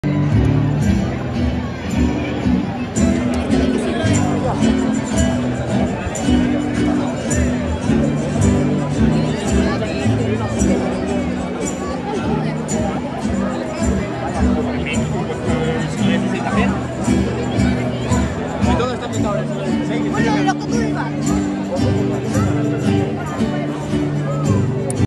Slackline en La Plaza de España en el Día del Corsario 2022
Corsarios sobre la ciudad de Santa Cruz de La Palma!

Día del Corsario. 18:00 del 4 de agosto de 20202.

En 1553 François Leclerc, corsario francés, junto a su lugarteniente Jacques de Soré llegaron a La Palma para arrasarla y quemarla.
Con el "Día del corsario" se quiere dar a conocer esta historia y enseñar que con el empeño y esfuerzo se pueden conseguir grandes cosas.

Donde se ha subido este vídeo es mi canal secundario donde subo cosas aleatorias y de menos calidad. Para ver mejores cosas: